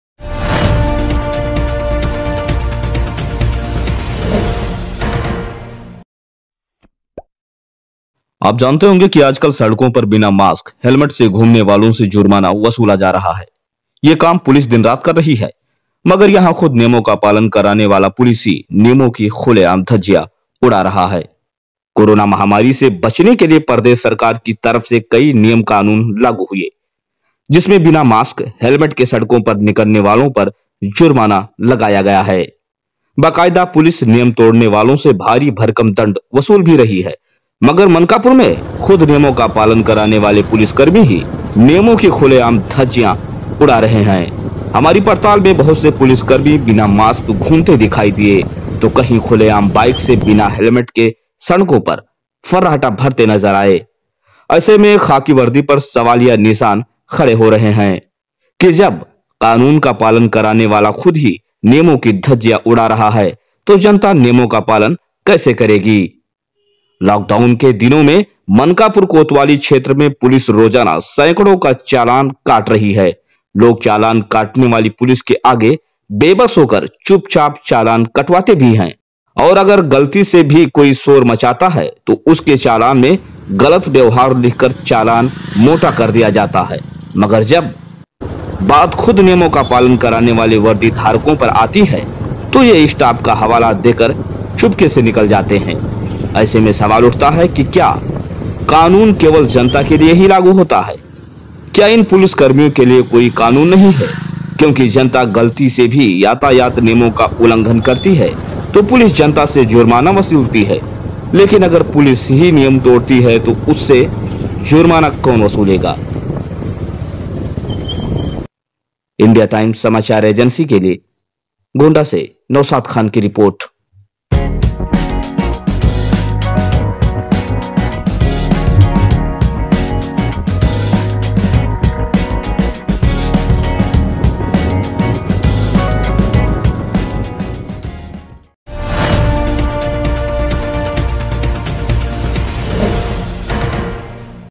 Mankapur Police : Police का कब कटेगा चालान
नियम-कानून का पाठ पढ़ाने वाले मनकापुर पुलिस ही उड़ा रहें हैं नियमों की धज्जियां

मनकापुर गोंडा / सड़कों पर बिना मास्क,हेलमेट के घूमने वालों से जुर्माना वसूला जा रहा है। मगर यहां खुद नियमों का पालन कराने वाली पुलिस ही नियमों की खुलेआम धज्जियां उड़ा रही है कोरोना माहमारी से बचने के लिए प्रदेश सरकार की तरफ़ से कई नियम लागू की गयी है।जिसमे बिना मास्क,हेलमेट के सड़कों पर निकलने वालों पर जुर्माने लगा रखा है।बाकायदा पुलिस नियम तोड़ने वालों से भारी भरकम दंड वसूल भी रही है।मगर यहां मनकापुर में खुद नियमों का पालन कराने वाले पुलिसकर्मी ही नियमों की खुलेआम धज्जियां उड़ा रहें हैं।